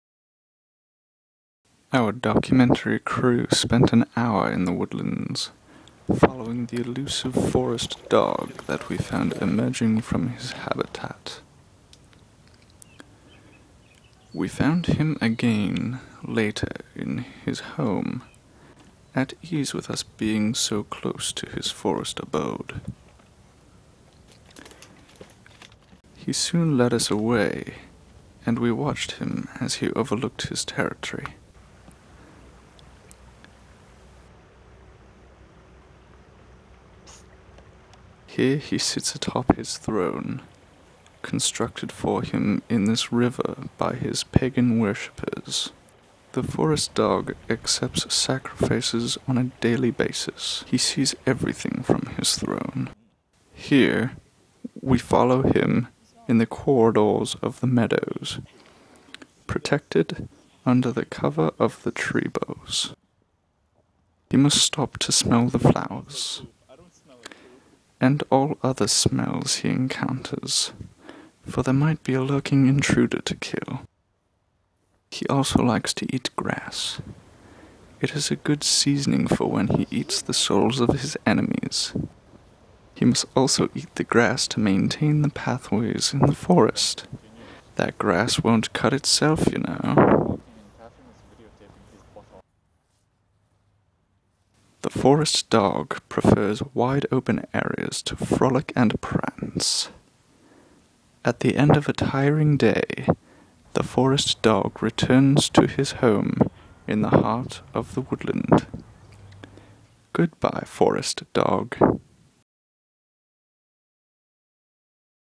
Tarqumentary
A silly "documentary" I made with my dog on a day off. Narrated by my brother.